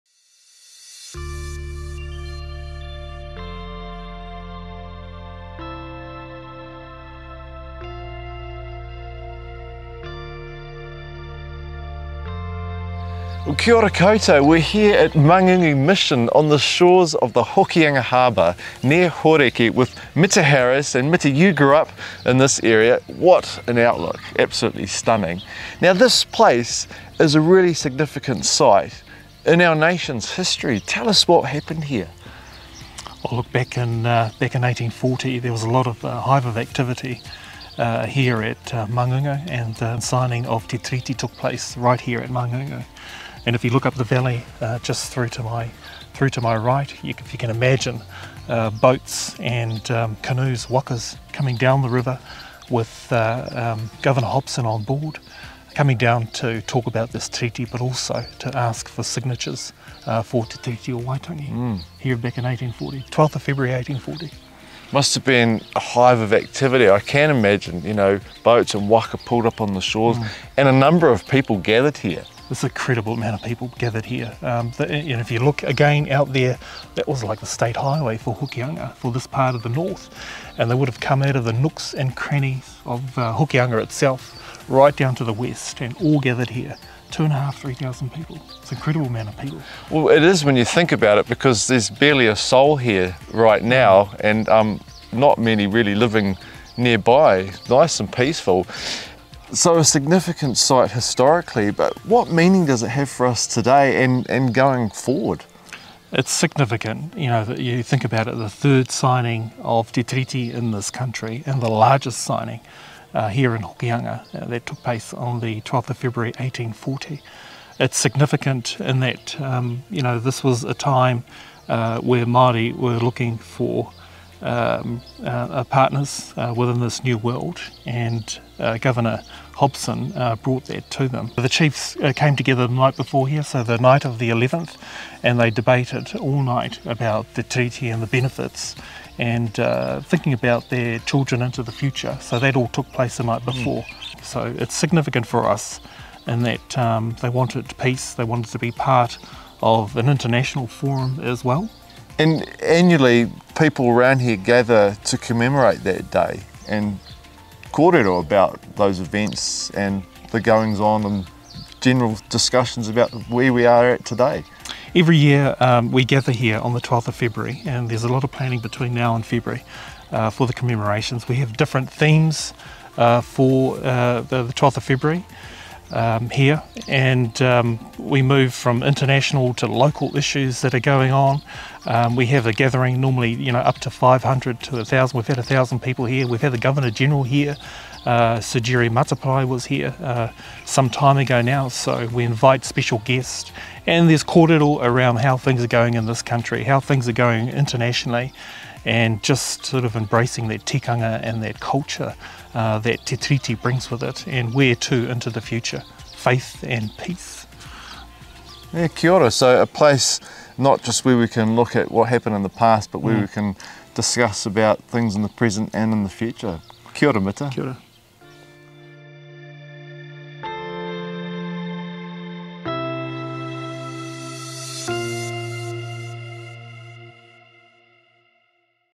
Māngungu and the signing of Te Tiriti
Māngungu Mission is a nationally significant site. It was where the largest ever signing of Te Tiriti o Waitangi took place. On February 12, 1840, a crowd of nearly 3000, representing many different hapū within Ngāpuhi, watched over 60 rangatira sign the document. So what does this location mean for people, both locally and nationally, and both historically and today going forward?